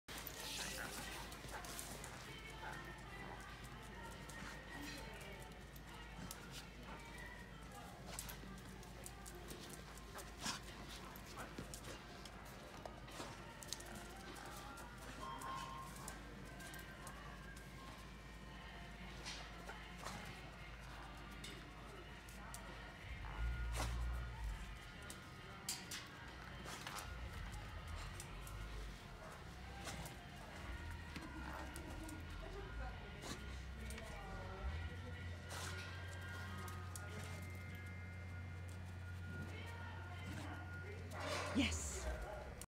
Entlebucher Mountain Dog Jaylah- training, learning about inaccessible hides.
Entlebucher Mountain Dog Jaylah (January 2020) nosework training. I set up an inaccessible hide (or I thought so LOL) behind the white wire shelving units in Lowe's. Jaylah is learning how to work through this scent puzzle, source and indicate when she is confident she found the hide. I imagined she would alert from the outside of the shelves but she pushes in behind to get to source. Good girly!